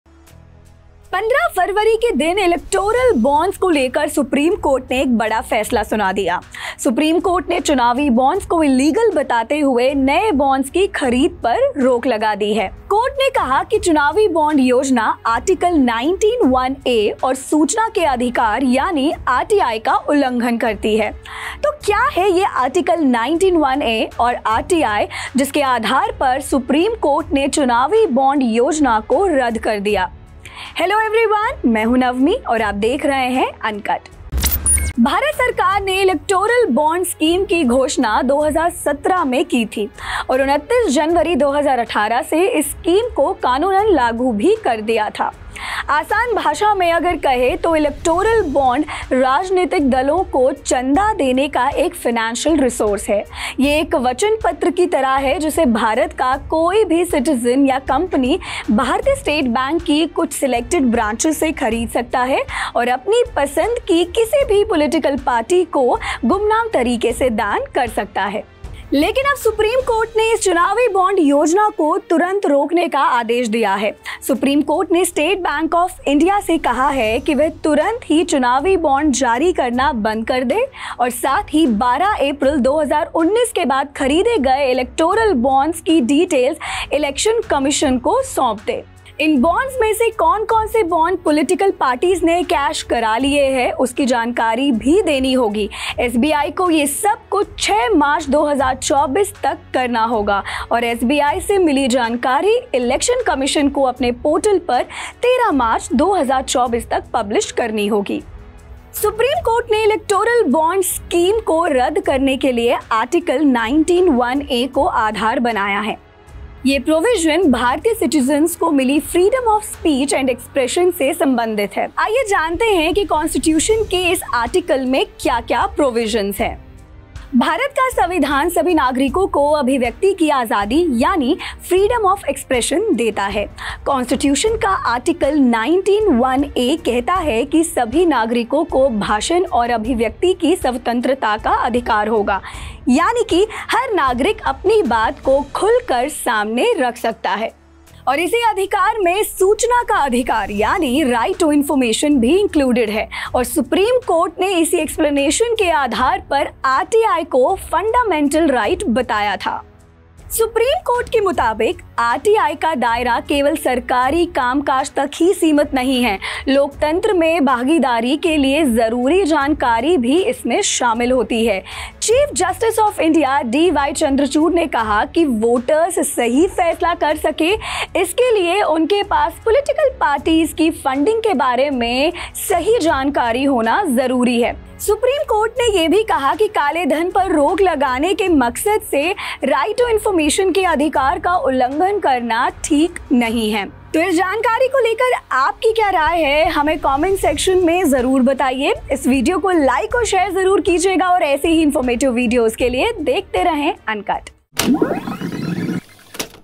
क्या है संविधान का article 19 (1) (a) और RTI जिसके आधार पर रद्द हुई Electoral Bond Scheme ?
Electoral Bond Scheme: क्या है संविधान का article 19 (1) (a) और RTI जिसके आधार पर रद्द हुई  चुनावी बॉन्ड योजना ?

15 फ़रवरी के दिन इलेक्टोरल बांड्स को लेकर सुप्रीम कोर्ट ने एक बड़ा फैसला सुना दिया।  सुप्रीम कोर्ट ने चुनावी बांड्स को illegal बताते हुए नए बांड्स की खरीद पर रोक लगा दी है।
कोर्ट ने कहा की चुनावी बॉन्ड योजना Article 19(1)(a) और सूचना के अधिकार यानि RTI का उल्लंघन करती है। तो क्या है ये Article 19(1)(a) और RTI जिसके आधार पर सुप्रीम कोर्ट ने चुनावी बॉन्ड  योजना को रद्द कर दिया, आइए जानते है Uncut की इस Video में।